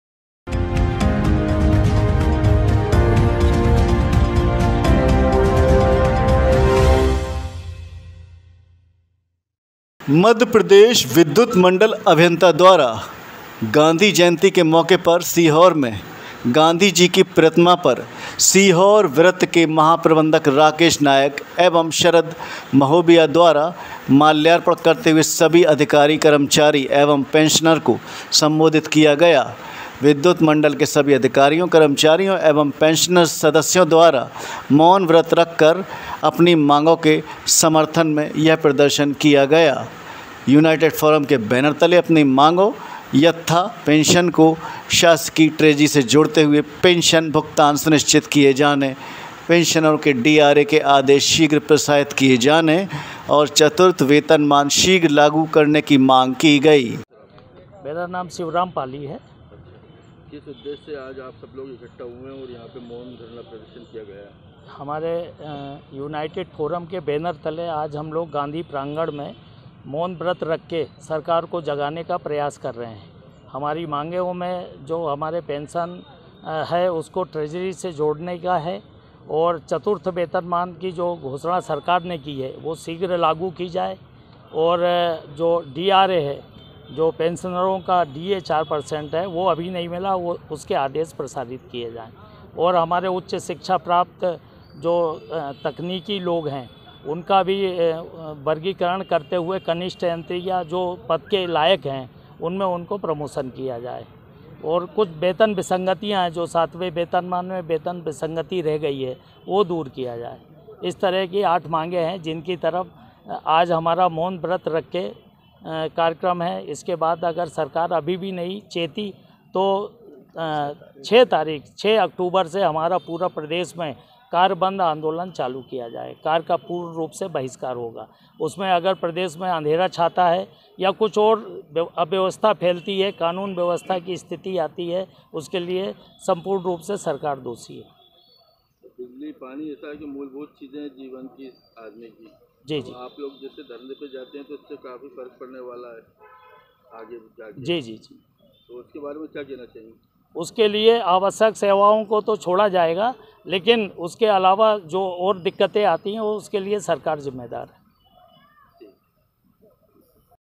सीहोर : गांधी प्रतिमा के सामने विद्युत कर्मचारियों का अपनी मांगों के संबंध में मोनव्रत
यूनाइटेड फोरम के बैनर तले अपनी मांगों यथा पेंशन को शासकीय ट्रेजरी से जोड़ते हुए पेंशन भुगतान सुनिश्चित किया जाने, पेंशनरों के डीआर के आदेश शीघ्र प्रसारित किये जाने तथा चतुर्थ वेतनमान शीघ्र लागू करने की मांग की गई। संविदा नियमितिकरण एवं सुधार उपरांत वर्ष 2023 संविदा नीति लागू की जावे।